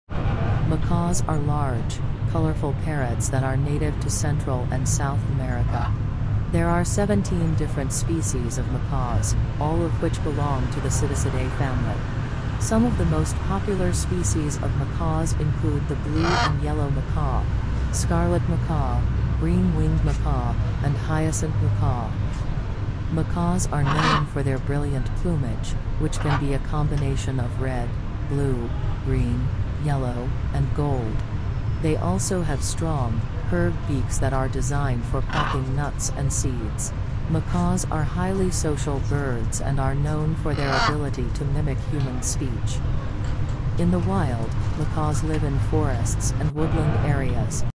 Macaws are large, colorful parrots /මැකෝ යනු විශාල වර්ණවත් ගිරවුන්/17 different species of macaws,
මධ්‍යම සහ දකුණු ඇමරිකාවට ආවේණික වූ විශාල වර්ණවත් ගිරවුන් වේ. මැකෝ විශේෂ 17 ක් ඇති අතර, ඒවා සියල්ලම Psittacidae පවුලට අයත් වේ. වඩාත් ජනප්‍රිය මැකෝ විශේෂ අතරට නිල් සහ කහ මැකෝ, තද රතු මැකෝ, කොළ පියාපත් ඇති මැකෝ සහ හයසින්ත් මැකෝ ඇතුළත් වේ.

My goal is to Watching activities very sensitive and taking them to the world